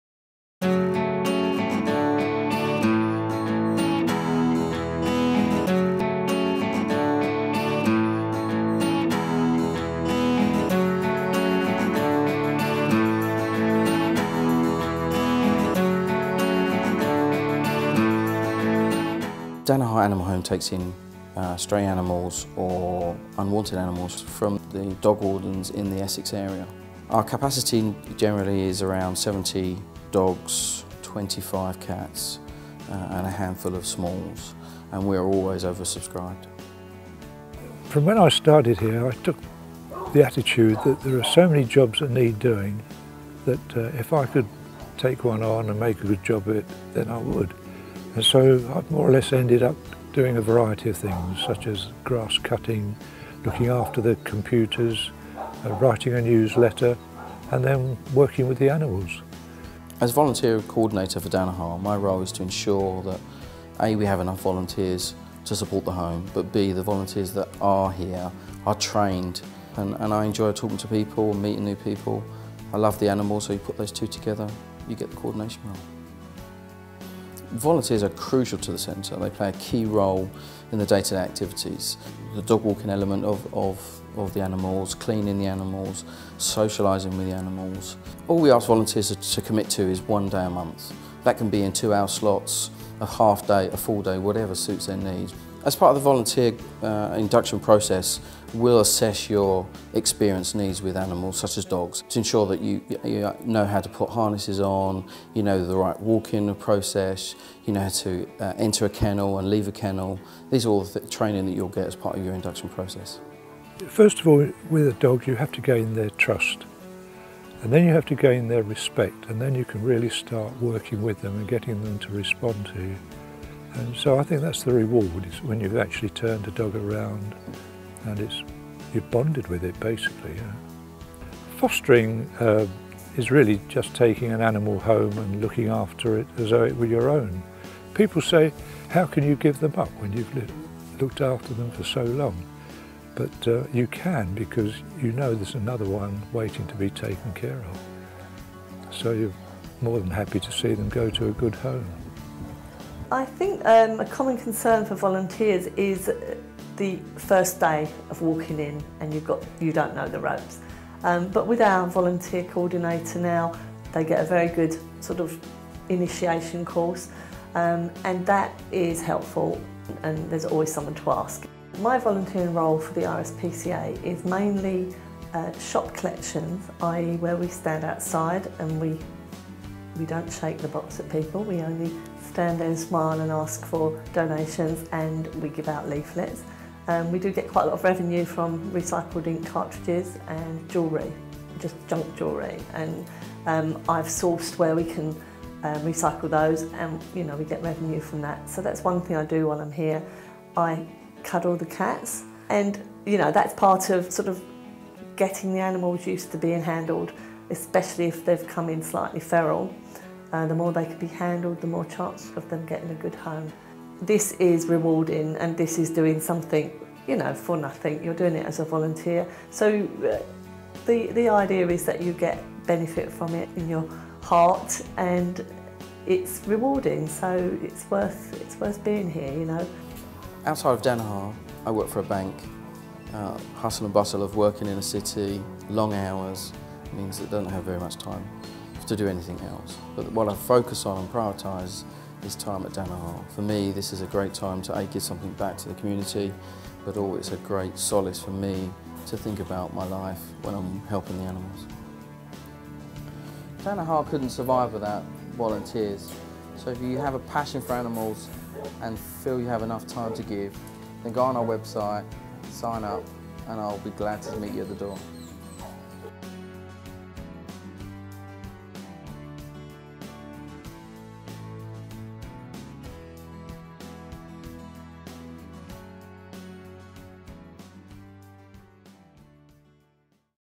Volunteering at Danaher Animal Home
Feel you have what it takes to volunteer at Danaher? sign up